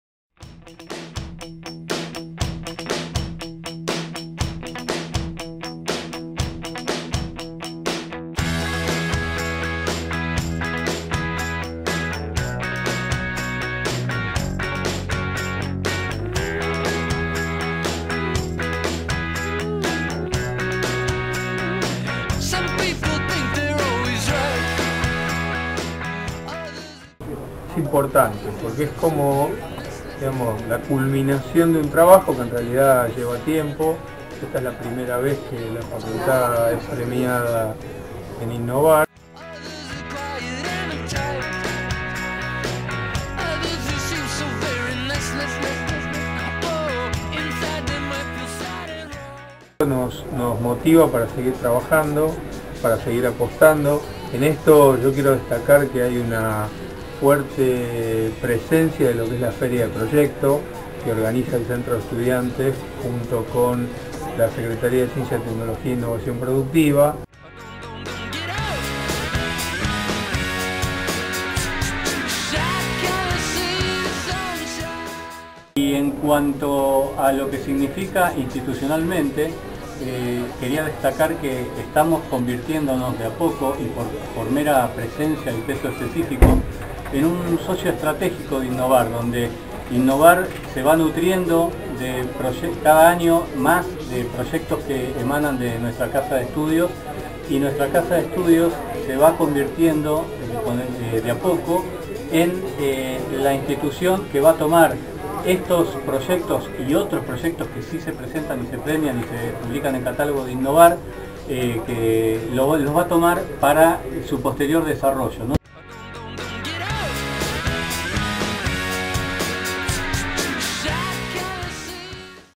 INNOVAR 2010 - UTN Facultad Regional Buenos Aires
Doble premio para la UTN Buenos Aires en INNOVAR 2010; en la categoría "Producto Innovador" fue distinguido el Proyecto Aprovechamiento de la Energía Undimotriz, mientras que en la categoría "Tecnologías para el Desarrollo Social" se premió el Proyecto de Solmáforo.